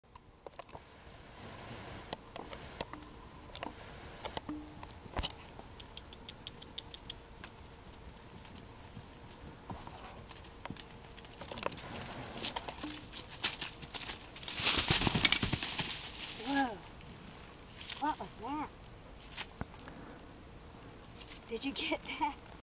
nissi spook
Neisti spooks when a critter goes thru the dry leaves in the bushes under a tree;.  he has never spooked before.